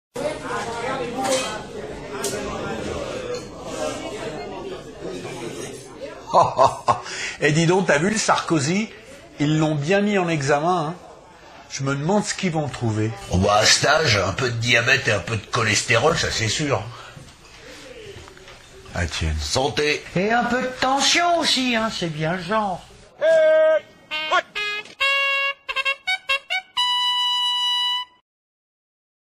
Brèves de comptoir - A partir d'un certain age...
Au bistro aujourd'hui on commente la mise en examen de Nicolas Sarkozy ;-)